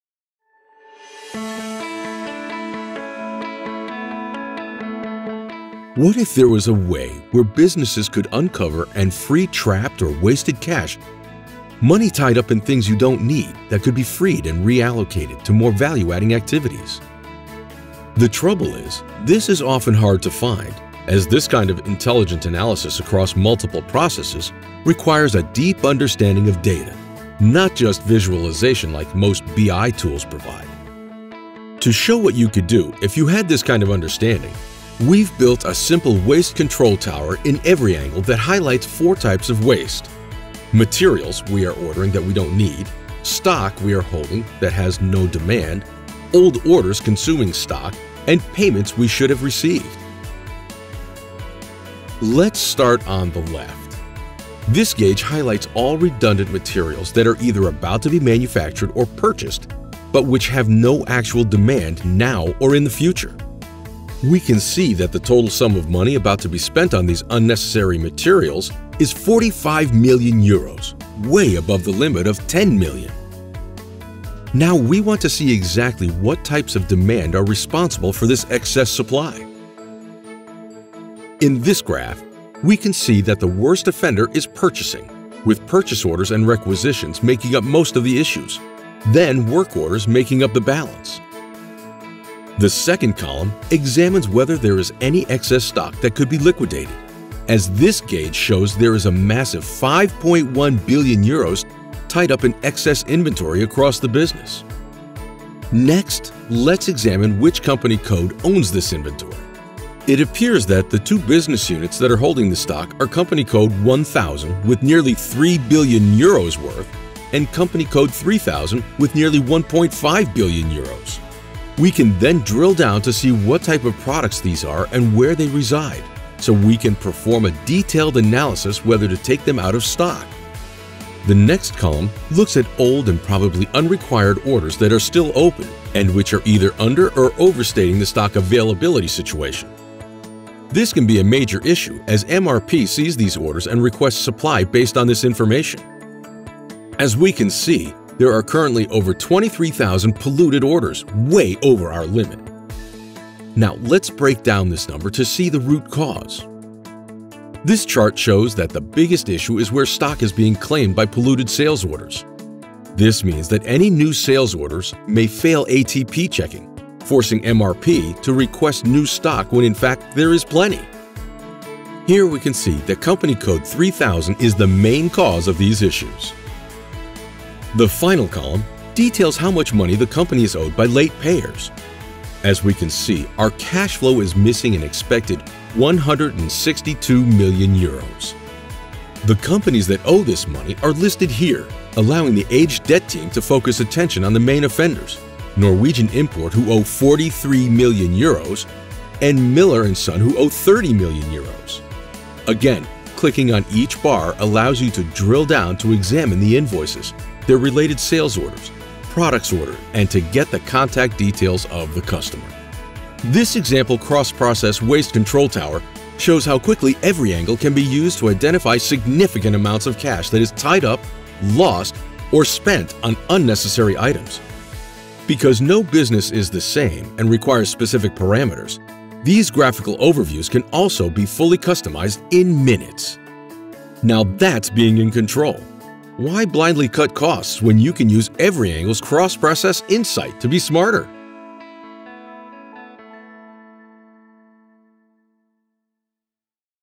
Every Angle Control Tower for SAP - Waste
What if there was a way where businesses could uncover and free trapped or wasted cash in your business? Money tied up in things you don’t need that could be freed and re-allocated to more value adding activities. The trouble is, this is often hard to find as this kind of intelligent analysis across multiple processes requires a deep understanding of data, not just visualization like most BI [bee-eye] tools provide.

To show what you could do if you had this kind of understanding, we’ve built a simple Waste Control Tower in Every Angle that highlights four types of waste – materials we are ordering that we don’t need, stock we are holding that has no demand, old orders consuming stock and payments we should have received.

This example cross-process waste control tower shows how quickly Every Angle can be used to identify significant amounts of cash that is tied up, lost or spent on unnecessary items. Because no business is the same and requires specific parameters, these graphical overviews can also be fully customized in minutes.

Now that’s being in control.  Why blindly cut costs, when you can use Every Angle’s cross-process insight to be smarter?